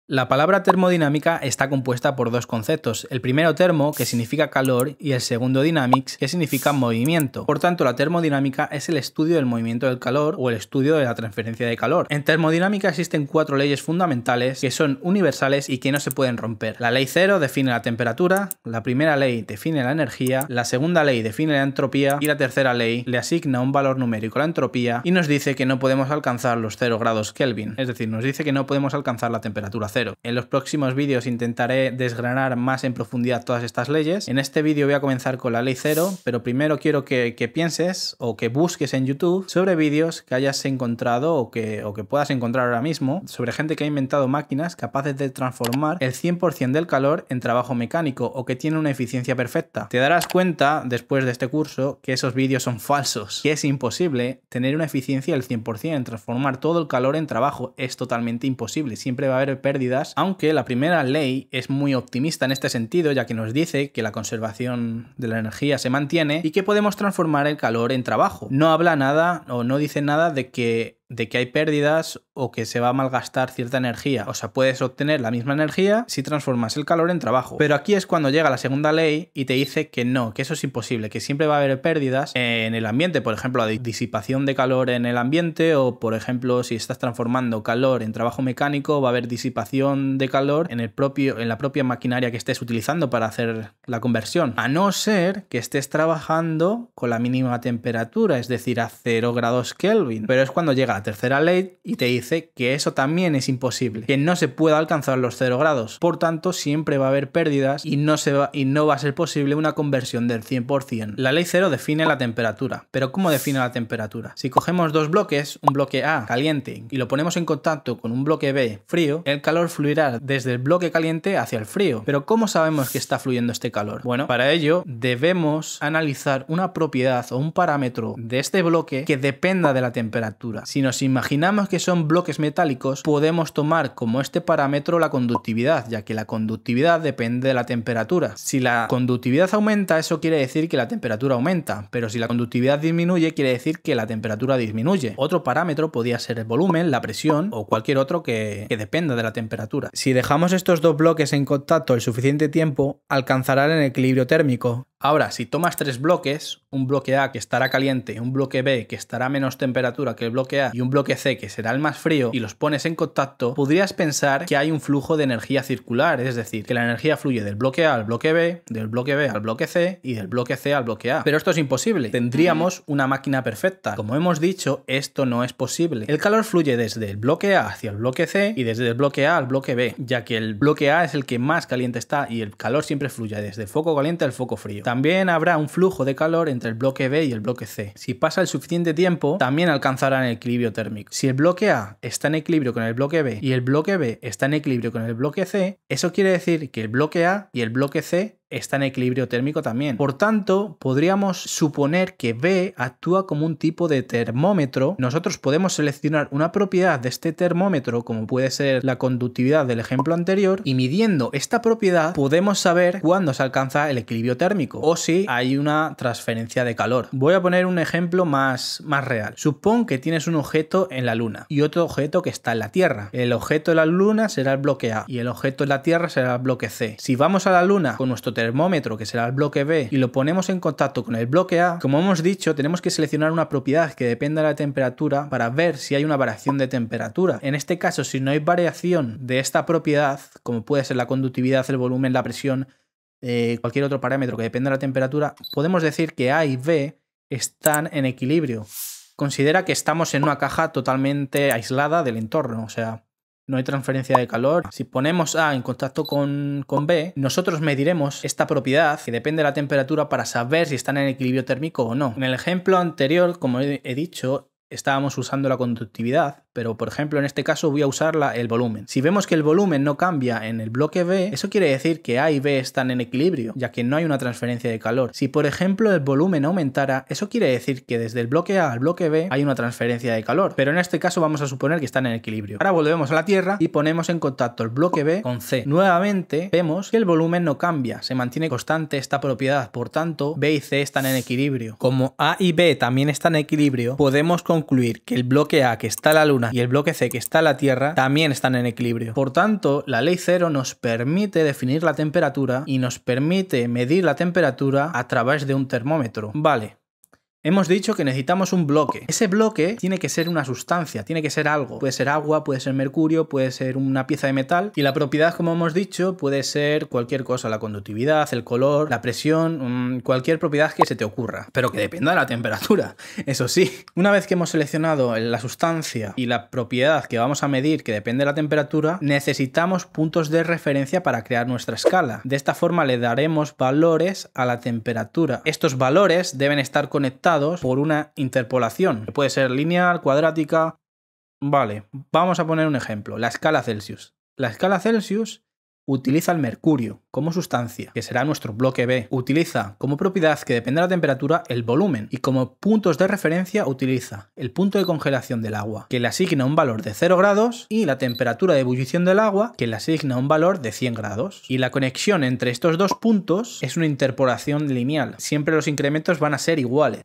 Curso de termodinámica: Introduccion y ley 0: Definicion de temperatura
Curso sobre termodinámica. Introducción y conceptos generales. Ley cero de la termodinámica.

Vídeo que recomiendo ver que es tiene una explicación más simple y concisa (y de hecho la diferencia entre centígrado y celsius no tenía ni idea)